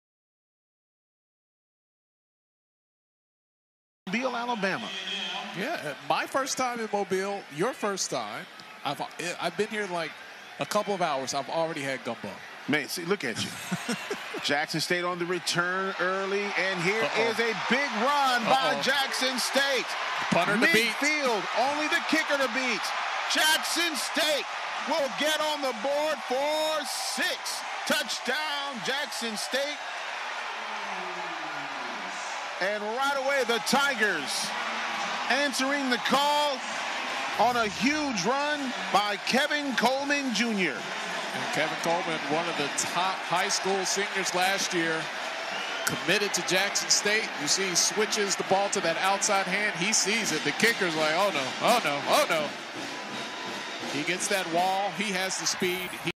JSU 4 STAR WR Kevin Coleman "FIRST" College Football Kick Return !
Unfortunately the kick return was called back.